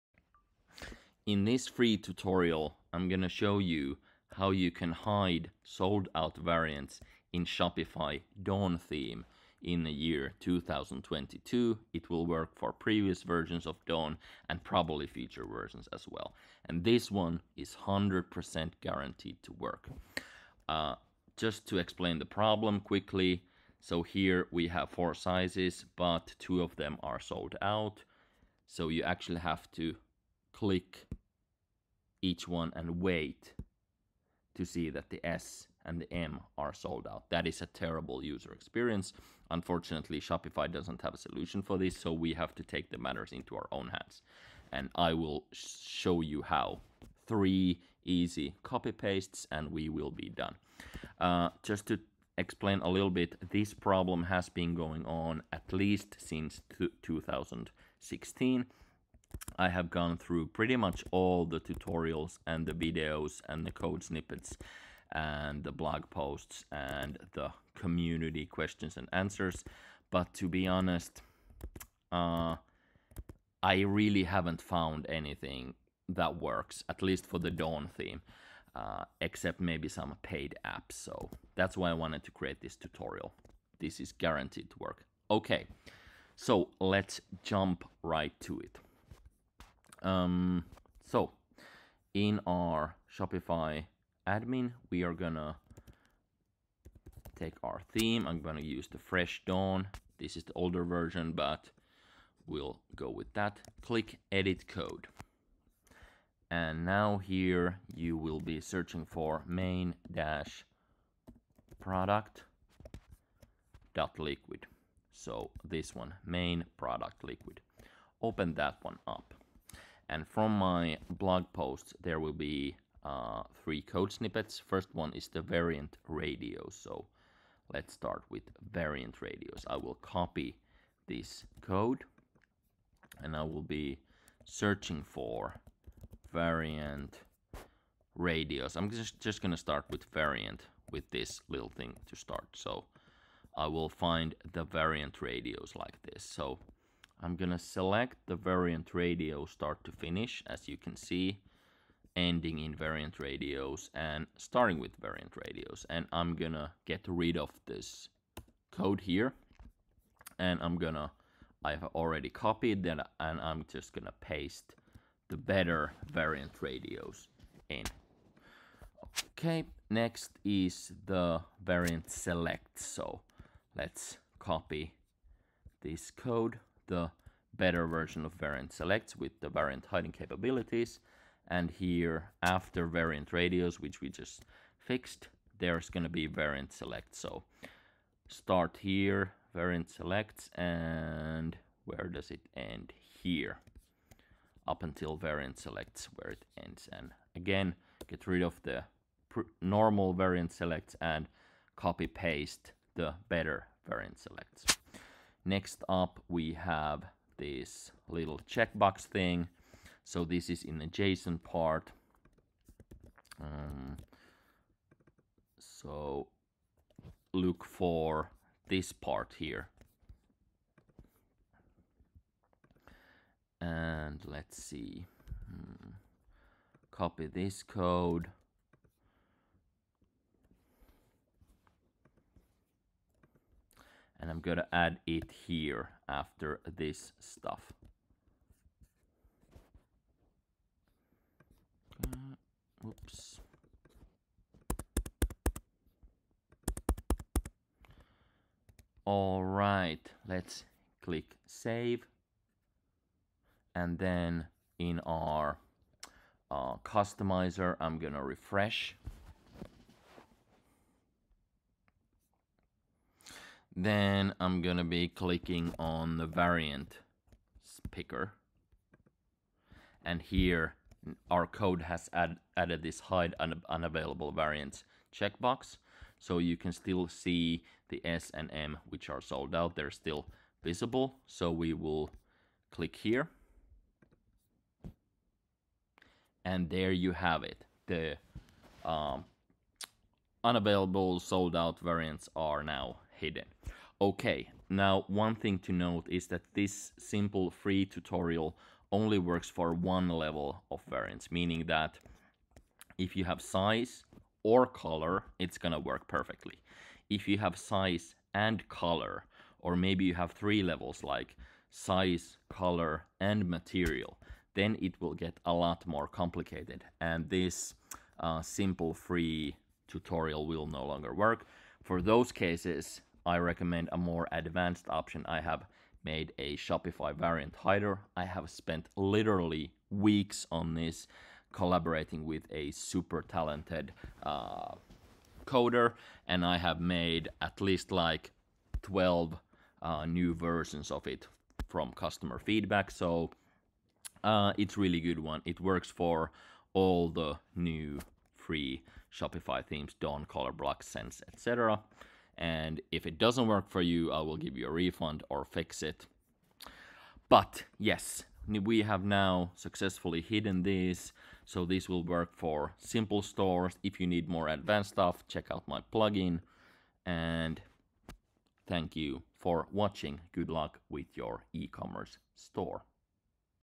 Hide Sold-Out variants in Shopify Dawn Theme · (BEFORE VERSIONS 8.0)
❗ AFTER UPDATE TO DAWN 8.0 or other free themes, this solution isn't compatible with new versions of those themes.